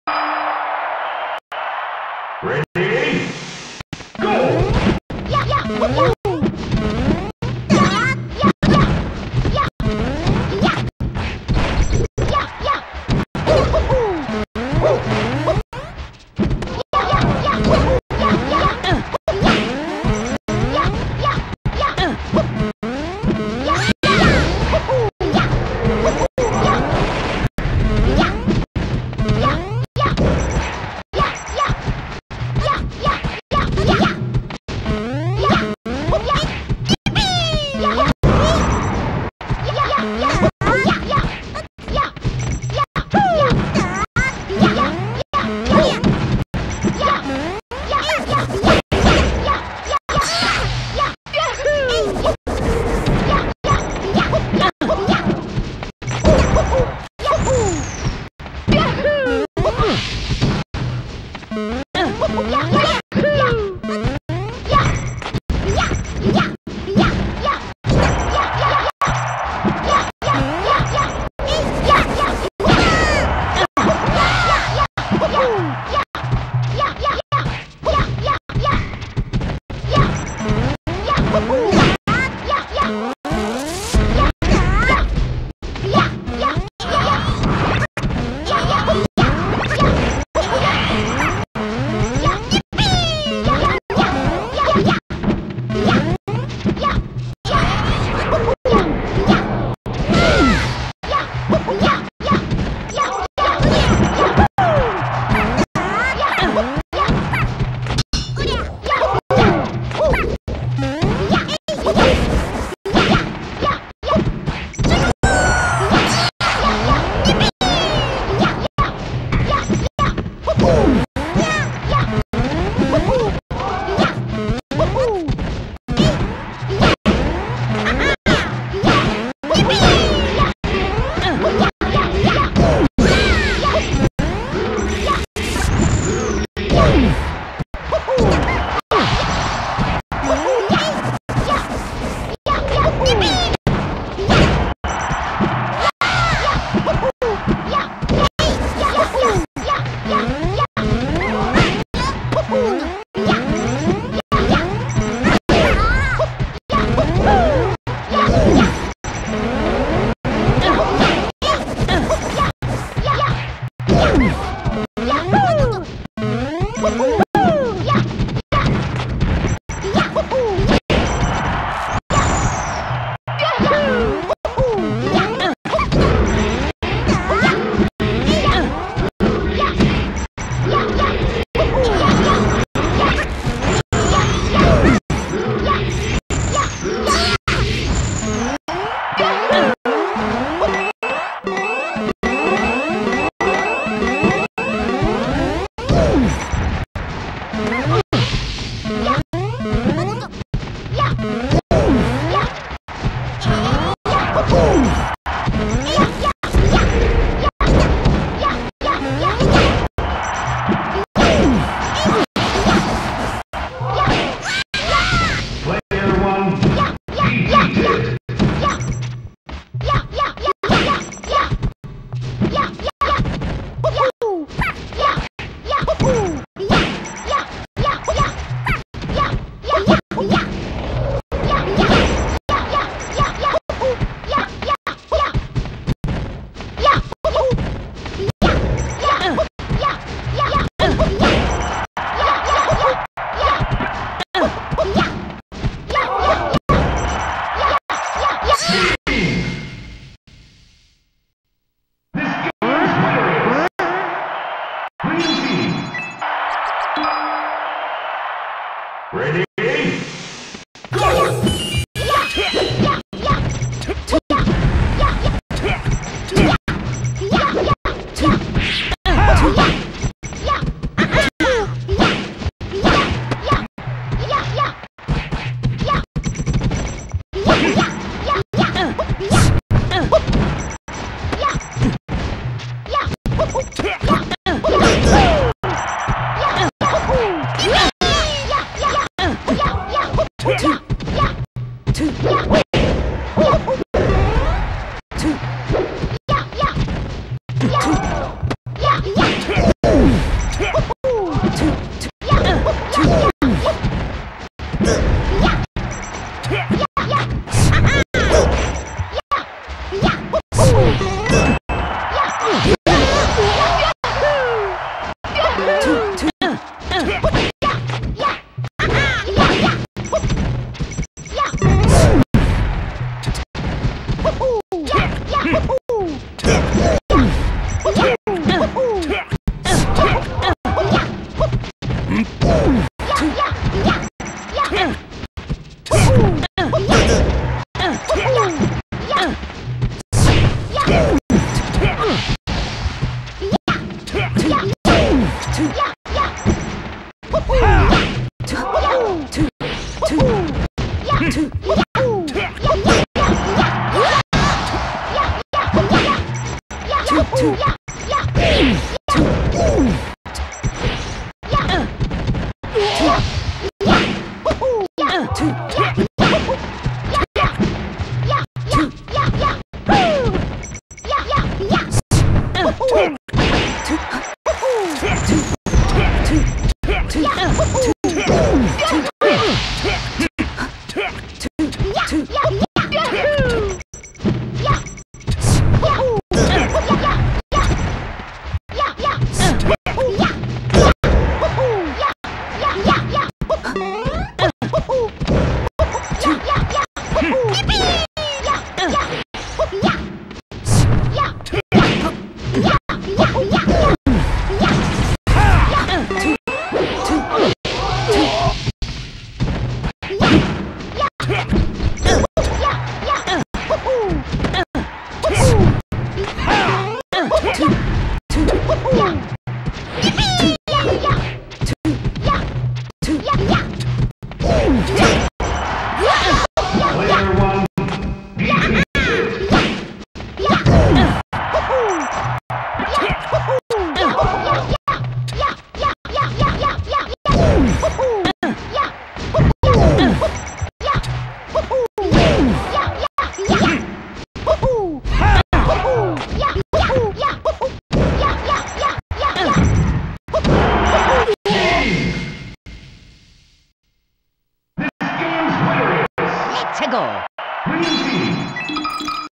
The Big House - Vanz (Luigi) & Vudujin (Luigi) vs Peef (ICs / Sheik) & The Number J (Puff / Ganon)
The Big House - 10/22/11
Ann Arbor, MI
Teams Winners Bracket Round 2